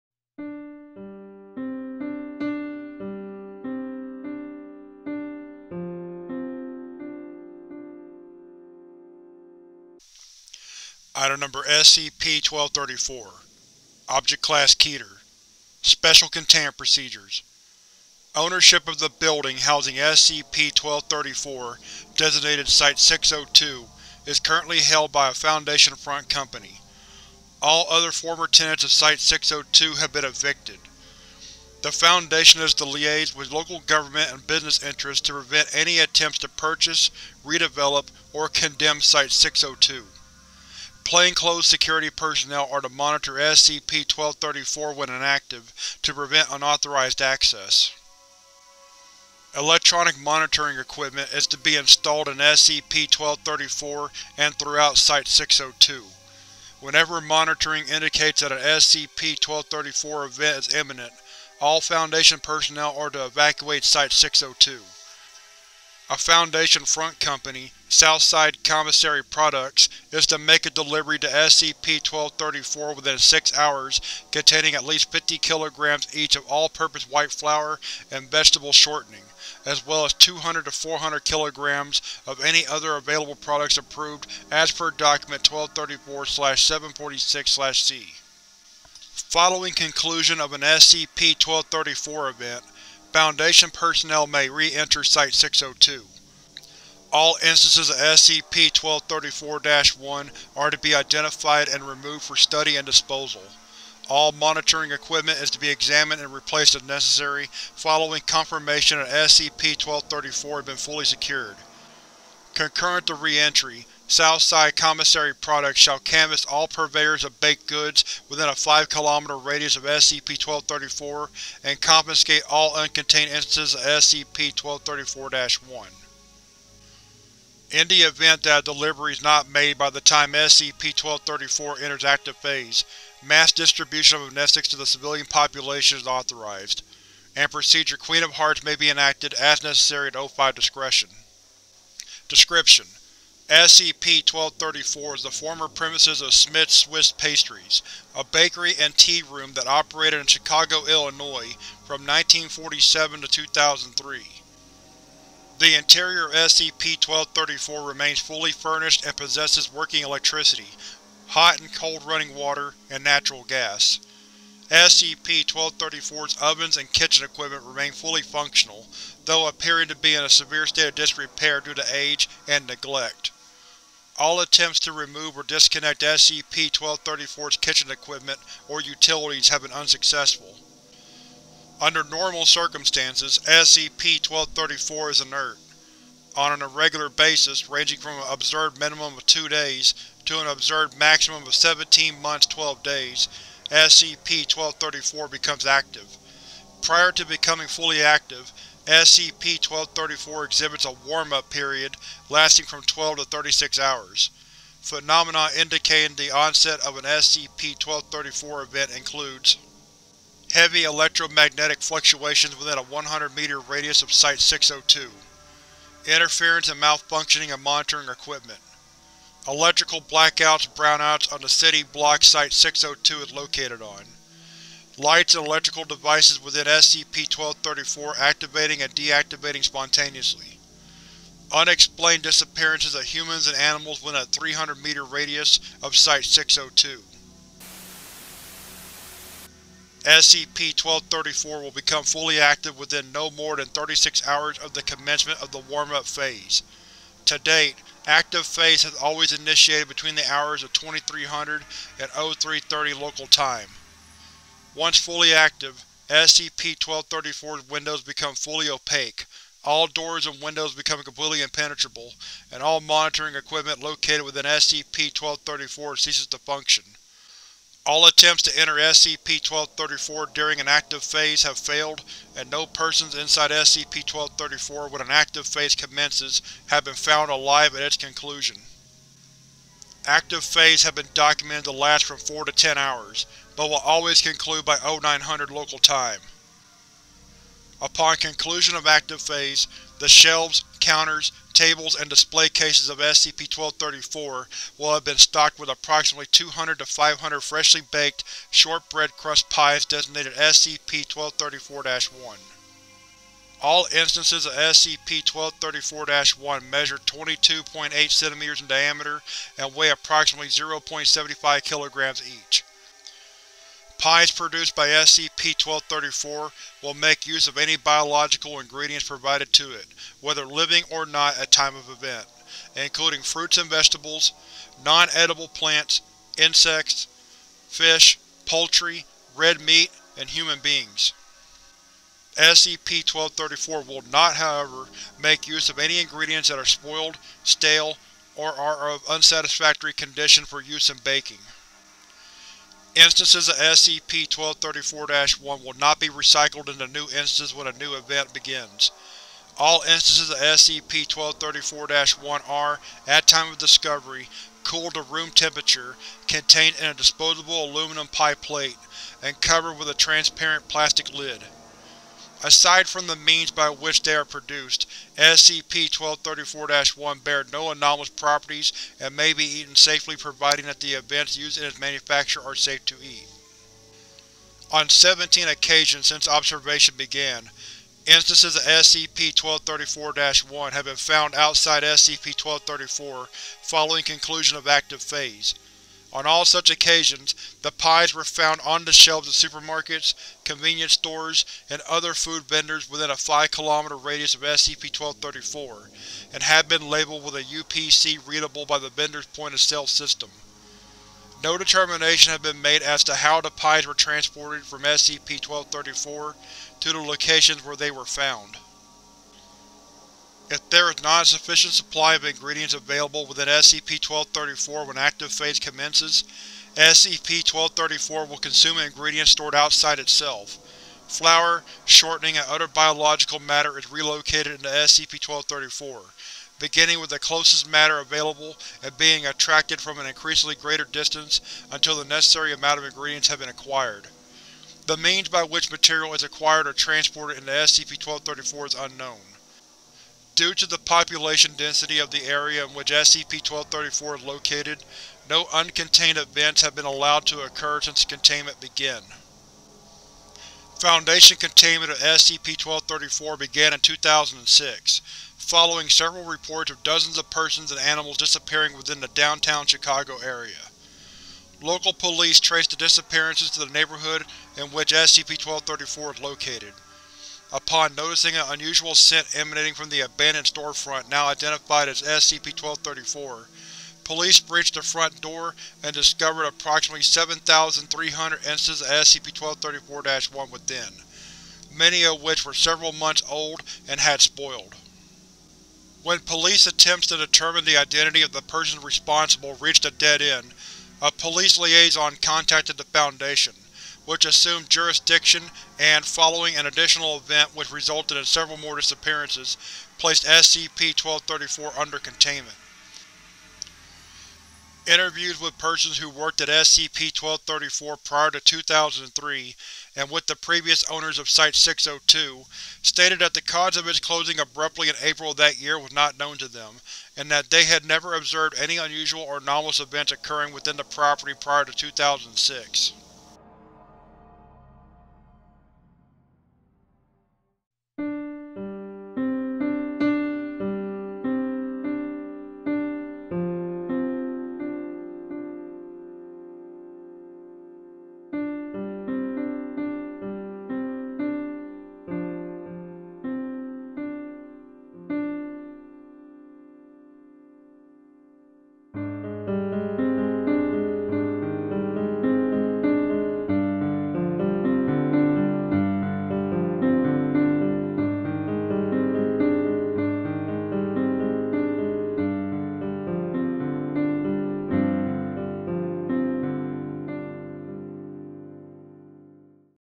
SCP-1234 An Abandoned Bakery - The Bakery That Bakes Everything (Including You)
This SCP Foundation wiki reading video is about SCP 1234 "An Abandoned Bakery". SCP Foundation object number SCP-1234 is the former premises of "Schmidt's Swiss Pastries", a bakery and tea room that operated in Chicago, IL from 1947 to 2003. The interior of SCP-1234 remains fully furnished and possesses working electricity, hot and cold running water, and natural gas. SCP-1234's ovens and kitchen equipment remain fully functional, though appearing to be in a severe state of disrepair due to age and neglect. All attempts to remove or disconnect SCP-1234's kitchen equipment or utilities have been unsuccessful.

SCP-1234 will become fully active within no more than 36 hours of the commencement of the warmup phase. To date, active phase has always initiated between the hours of 23:00 and 03:30 local time. Once fully active, SCP-1234's windows become fully opaque, all doors and windows become completely impenetrable, and all monitoring equipment located within SCP-1234 ceases to function. All attempts to enter SCP-1234 during an active phase have failed and no persons inside SCP-1234 when an active phase commences have been found alive at its conclusion.

Active phase has been documented to last from 4-10 hours, but will always conclude by 09:00 local time. Upon conclusion of active phase, the shelves, counters, tables, and display cases of SCP-1234 will have been stocked with approximately 200-500 freshly baked, shortbread-crust pies designated SCP-1234-1. All instances of SCP-1234-1 measure 22.8 cm in diameter and weigh approximately 0.75 kg each. Pies produced by SCP-1234 will make use of any biological ingredients provided to it, whether living or not at time of event, including fruits and vegetables, non-edible plants, insects, fish, poultry, red meat, and human beings. SCP-1234 will not, however, make use of any ingredients that are spoiled, stale, or are of unsatisfactory condition for use in baking. Instances of SCP-1234-1 will not be recycled into new instances when a new event begins. All instances of SCP-1234-1 are, at time of discovery, cooled to room temperature, contained in a disposable aluminum pie plate, and covered with a transparent plastic lid. Aside from the means by which they are produced, SCP-1234-1 bear no anomalous properties and may be eaten safely providing that the ingredients used in its manufacture are safe to eat.

Written by Smapti